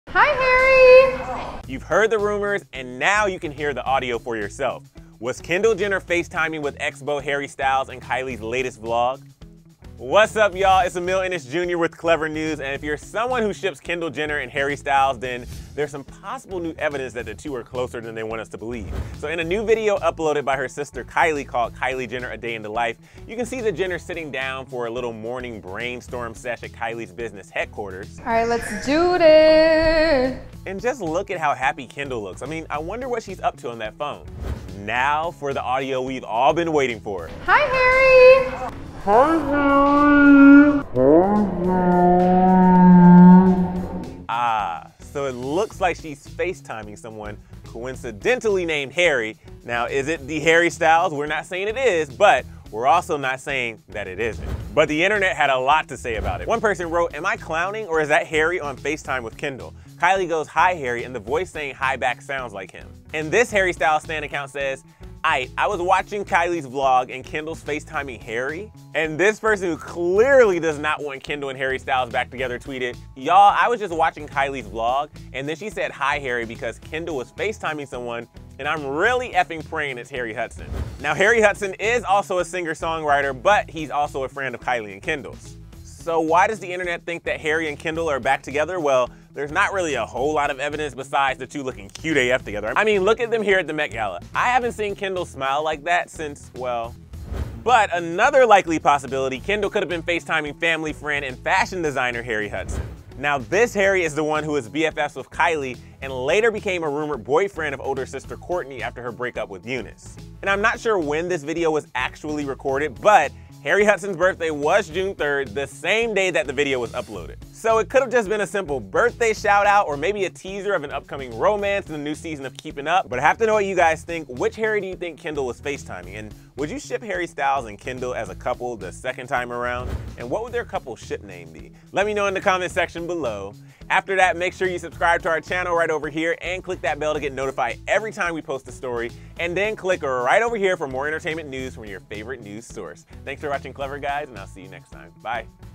Kendall Jenner FaceTimes With A 'Harry' And Fans Are FREAKING OUT It's Harry Styles!
You’ve heard the rumors. And now, you can hear the audio for yourself. Was Kendall Jenner FaceTiming with ex-beau Harry Styles in Kylie’s latest vlog???

What’s up y’all it’s Emile Ennis Jr. with Clevver News and if you’re someone who ships Kendall Jenner and Harry Styles, there’s some possible new evidence that the two are closer than they want us to believe.

In a new video uploaded by sister Kylie, called “Kylie Jenner: A Day in the Life” you can see the Jenners sitting down for a little morning brainstorm sesh at Kylie’s business headquarters.
Broll from video Kylie walking into meeting -

And look at how happy Kendall looks! I wonder what she’s up to on that phone
Freeze frame on video - show Kendall’s smile

Now for the audio we’ve all been waiting for…

Ahhh so it LOOKS like she’s facetiming someone, coincidentally named Harry. Is it THE Harry Styles…. We are not saying it is, but we are not saying that it isn’t.

AND the internet had a lot to say about it.

One person wrote,
“Am I clowning or is that harry on facetime with Kendall..Kylie goes “hi harry” and the voice saying hi back sounds like him”

This Harry Styles stan account said, “Aight I was watching Kylie’s vlog and kendall’s facetiming Harry?”

And this person who CLEARLY does NOT want Kendall and Harry Styles back together tweeted: “y’all i was just watching kylie’s vlog and then she said “hi harry” bc kendall was fting someone and i’m really f g praying it’s harry hudson.”

Harry Hudson is also a singer/songwriter, but is a friend of Kylie and Kendall’s.

So why does the Internet think the model and singer are back together?

Well, there’s really not a whole lot of evidence besides the two looking cute A F together.

I mean, look at them here at the MET gala! I haven’t seen Kendall smile like that since, well
But another likely possibility? Kendall could have been facetiming family friend and fashion designer Harry Hudson.

THIS Harry is the one who was bffs with Kylie and later became a rumored boyfriend of older sister Kourtney after her breakup with Younes.

And I’m not sure when this video was actually recorded, but Harry Hudson’s birthday was June 3, the same day that the video was uploaded.

So it could have been a simple birthday shout out, or maybe a teaser of an upcoming romance in the new season of Keeping Up….. but the jury is still out on that.

What do you guys think? Which Harry do you think Kendall was Facetiming? Would you ship Harry Styles and Kendall as a couple a second time around, and what would their couple SHIP name be?

Then click right over here for more entertainment news from your favorite news source.